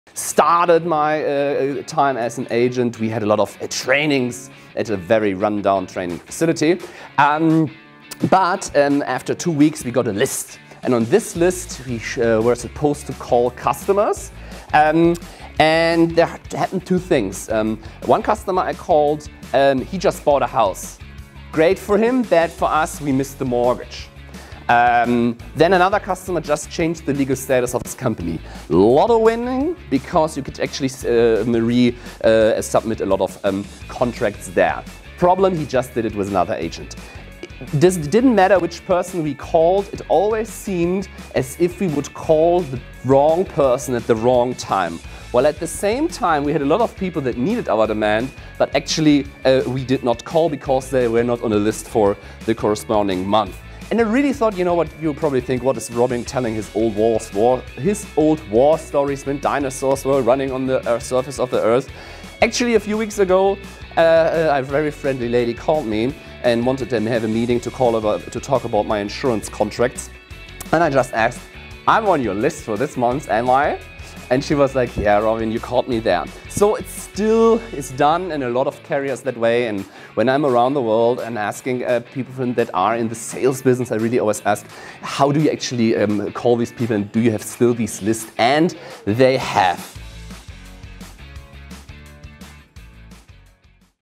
My first days as an Insurance agent
I really love hearing or watching stories from long ago.
Maybe some of you were fans of great stories from your grandfathers but not with the old insurance stories - correct?

But for me, I love them... Why? Because of these old tales and processes of insurance, you will realize what has to change to innovate the industry.
Same to old stories - you will know those old strategies will not work in today's technologies.

Similar to the old insurance marketing strategy - why use the same ancient process of having a calling list?
As mentioned, you also need to have the perfect timing just for you to offer your products and services to customers on your list.

We now have the internet, socialmedia, apps, voice, and other digitalmarketing automated methods - just leverage from the experts and let the system work for your business.